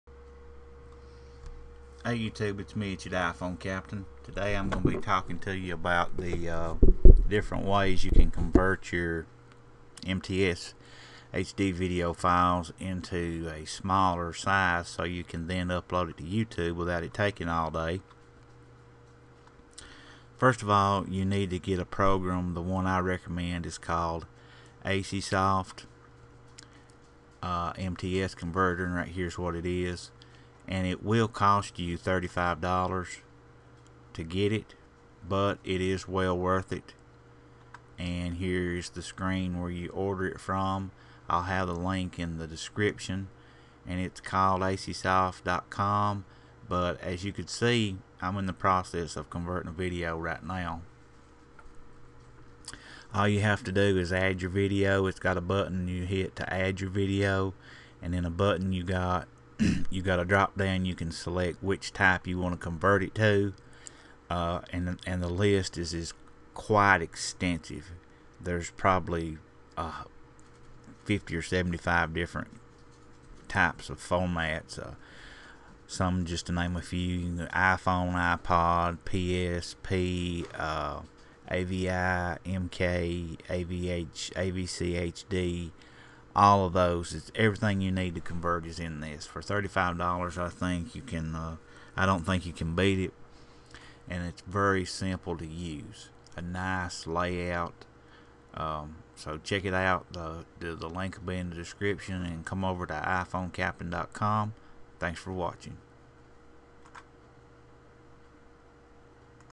Convert HD Video files MTS to AVI, AND MORE.......
for all the latest tips and tutorials for iphone, ipad, and ipod.  This video will show how to convert HD video MTS files to other formats such as AVI and more.